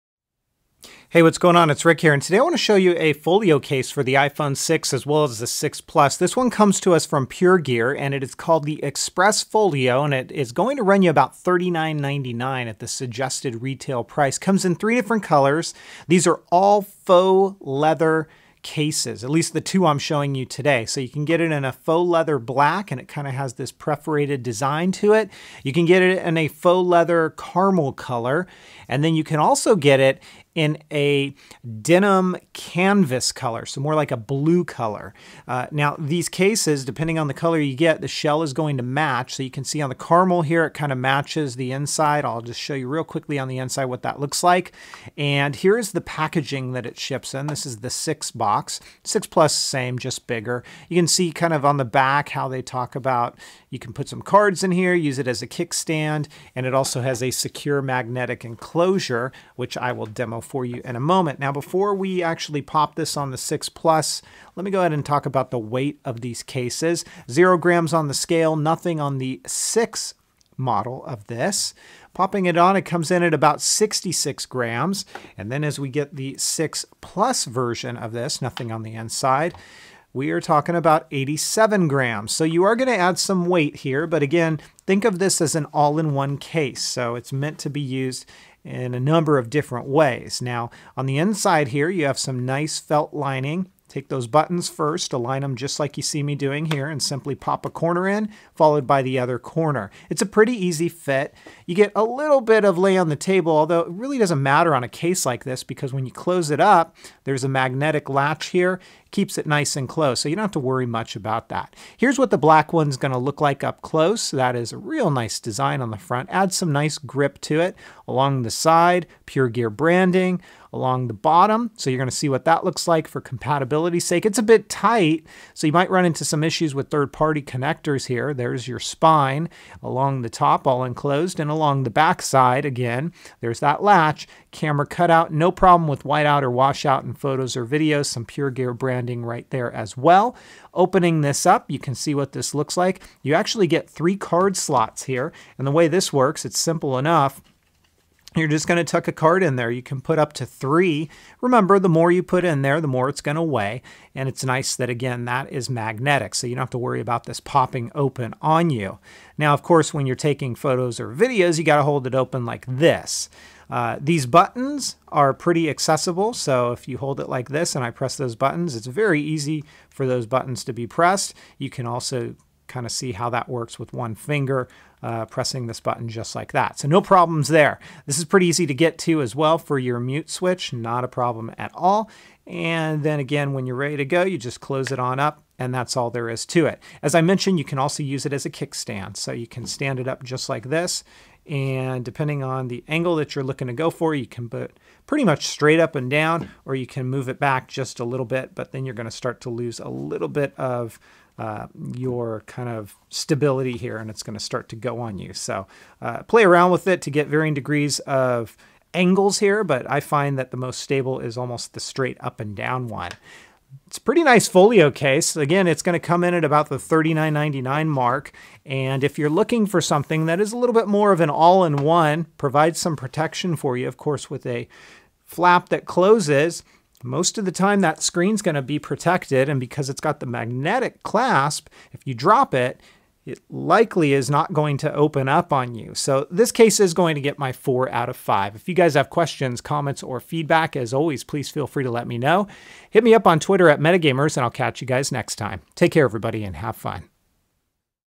PureGear Express Folio iPhone 6 and 6 plus case review
In this video I show you the PureGear Express Folio Case for the iPhone 6 and 6 plus. For more details on this product and to purchase please